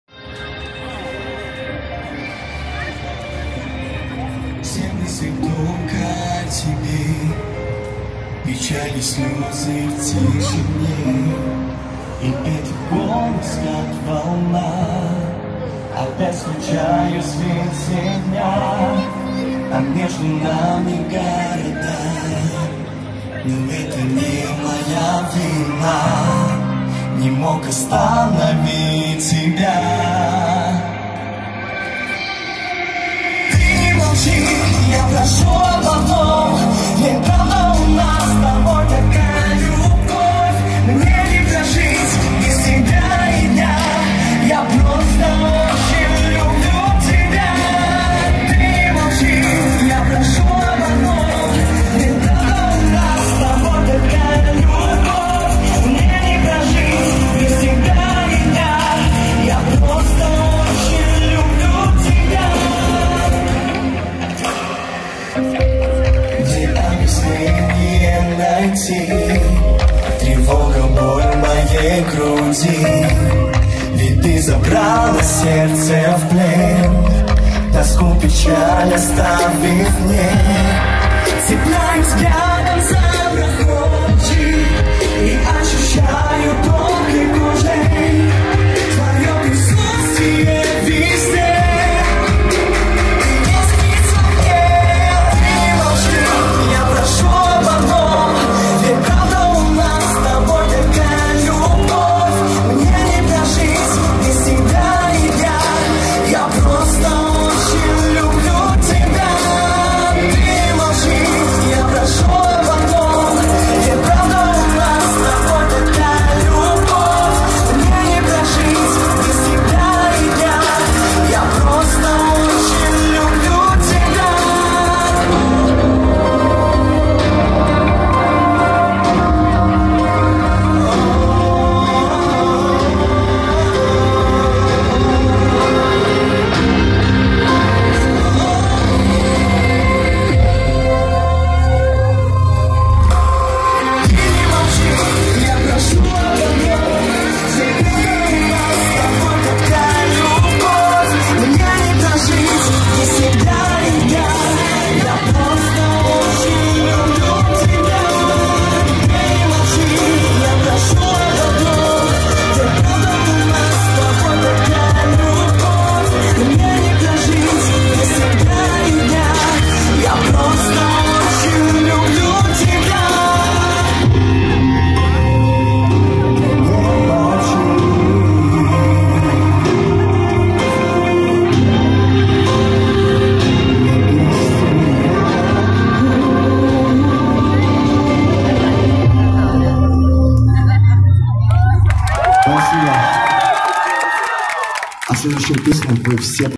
II Игры стран СНГ. Концерт на Немиге. 8.08. 2023 г. Культурная программа для участников и гостей игр
игры концерт культурная программа